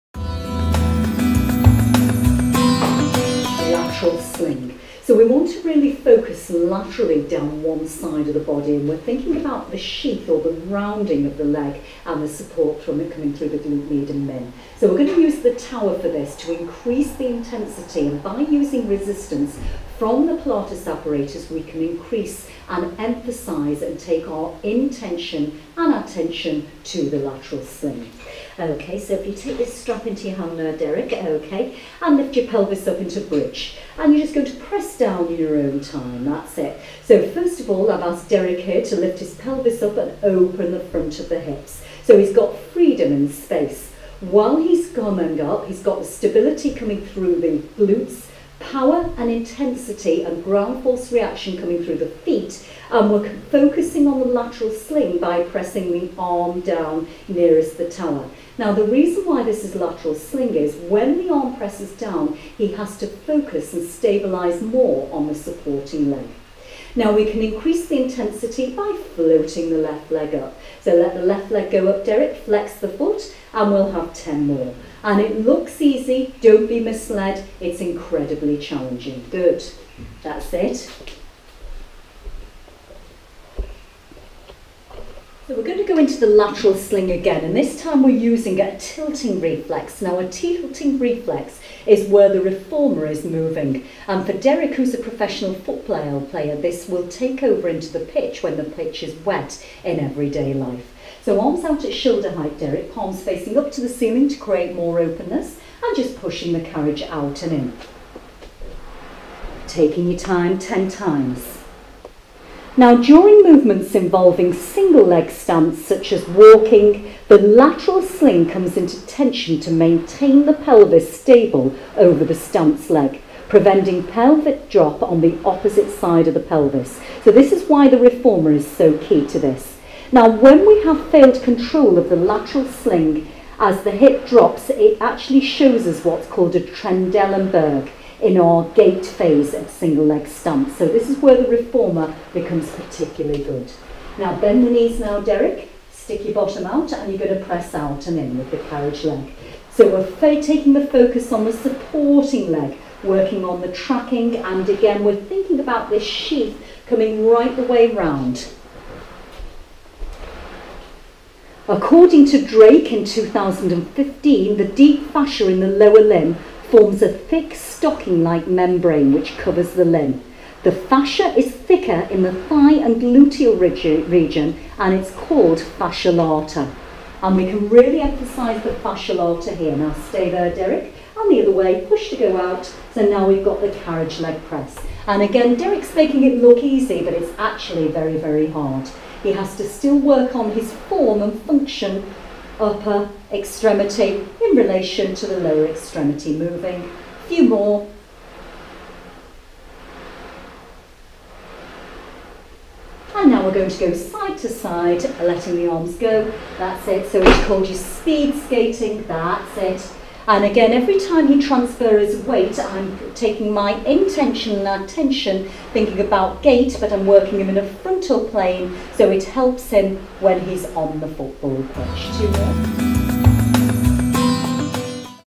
Lateral sling system - Myofascial slings (Vleeming & Lee)
The lateral sling consists of: gluteus medius, gluteus minimus, tensor fascia latae, iliotibial band.
According to Drake et al (2015), the deep fascia in the lower limb forms a thick ‘stocking-like’ membrane which covers the limb. The fascia is thicker in the thigh and gluteal region and is called the fascia lata.
Thank you to Derek Osede professional footballer for being the body in this.
This is a pilates perspective view of this sling system.